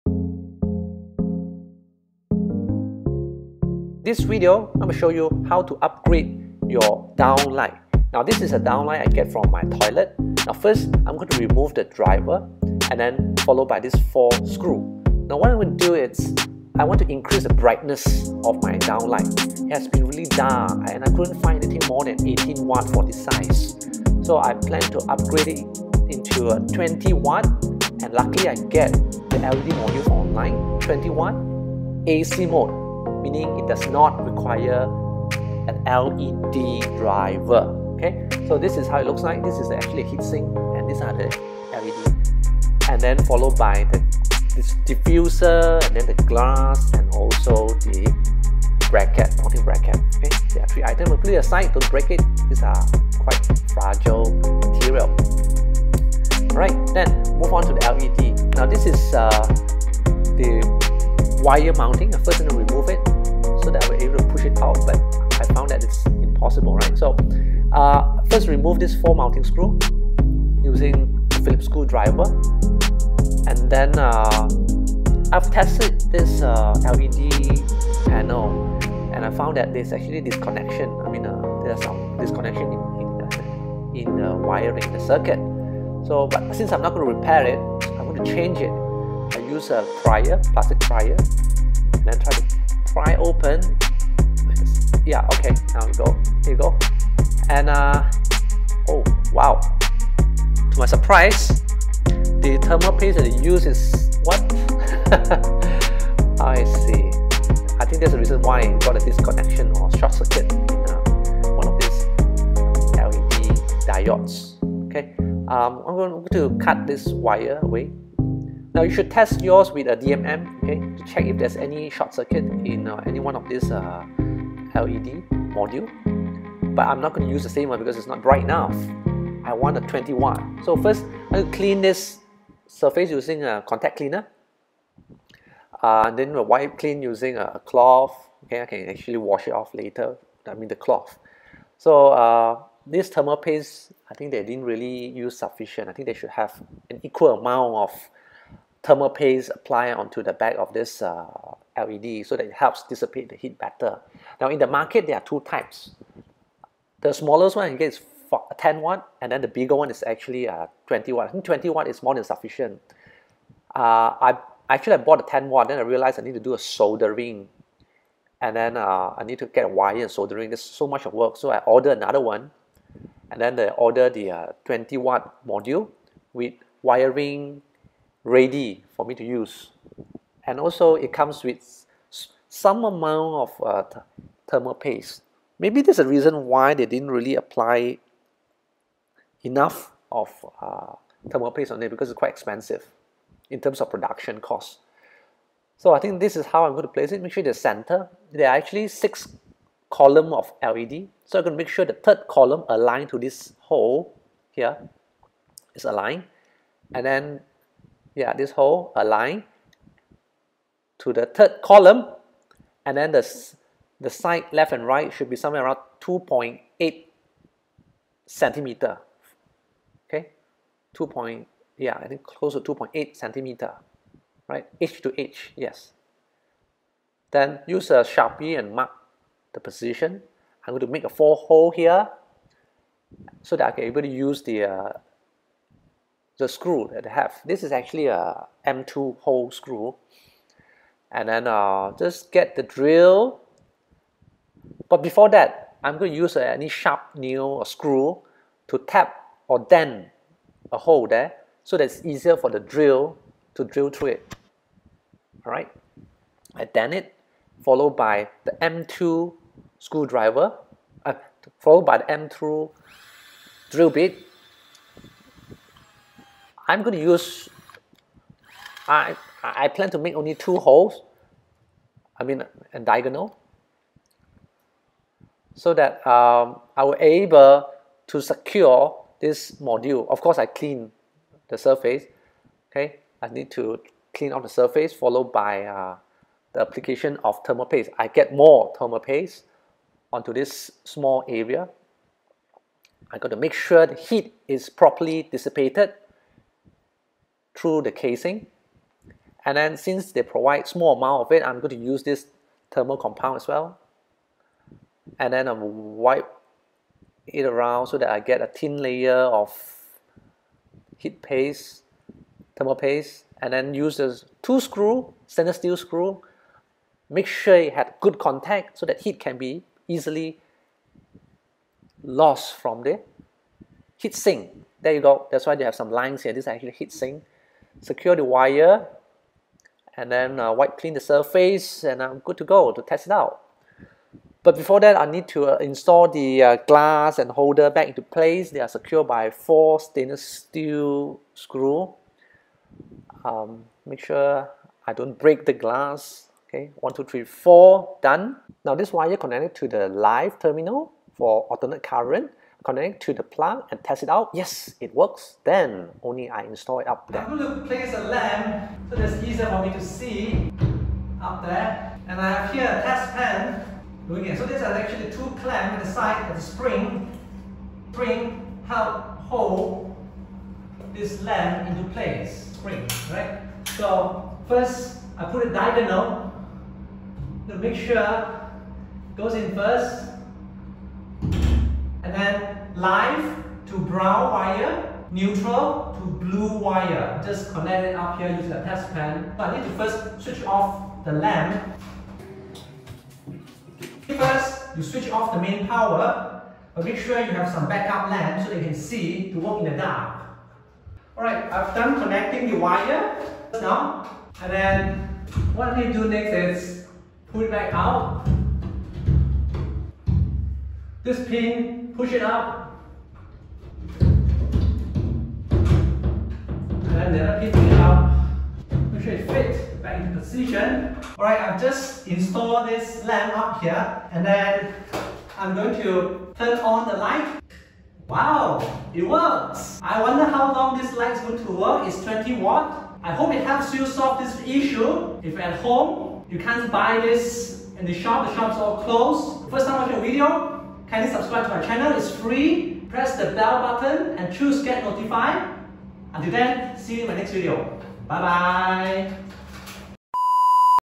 Stop trashing malfunction downlight |Upgrade it to 20W AC LED module using the same housing
This video show how to upgrade current 18W LED module to 20W AC LED module using the same housing without the use of LED driver.